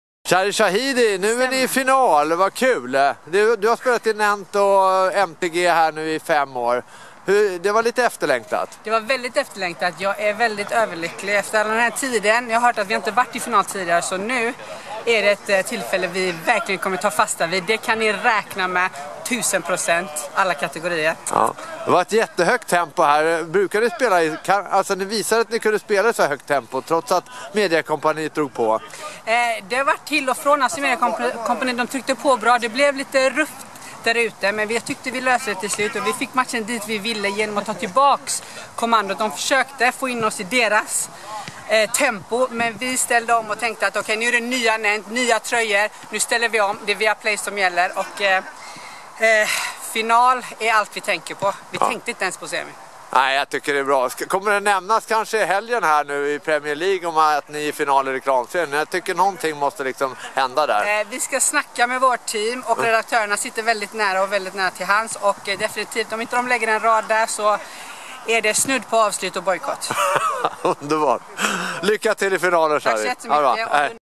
Reklamserien 2021 - Intervju Shari Shahidi
Spjuvern Shari Shahidi, den perfekta Reklamserieliraren, är mycket glad över finalplatsen och tycker att ett omnämnande bör ske på ViaSats PL-sändningar i helgen.